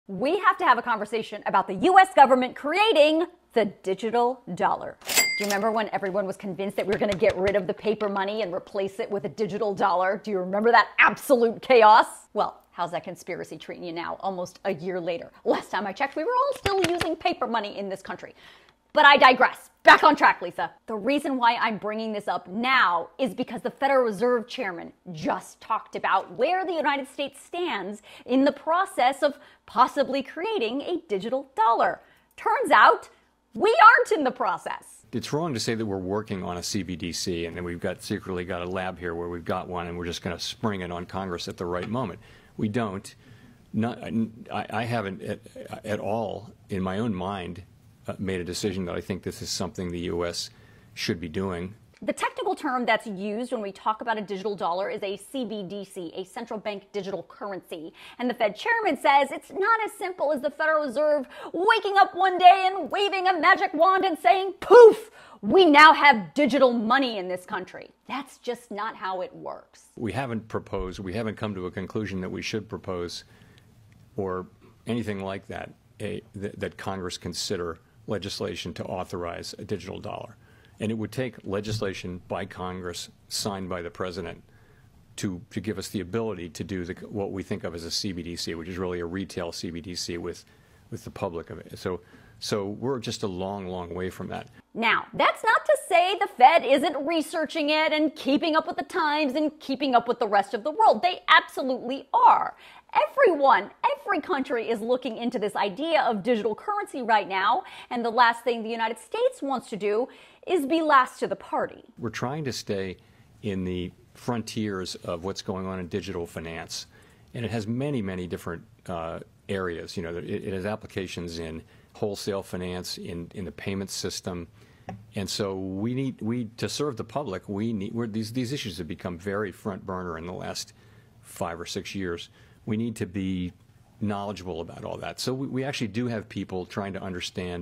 Is paper money going away? Fed chair discusses the digital dollar
The Federal Reserve Chairman just gave an update on where the United States is in the process of creating a digital dollar or a CBDC. This video explains what he said about the future of money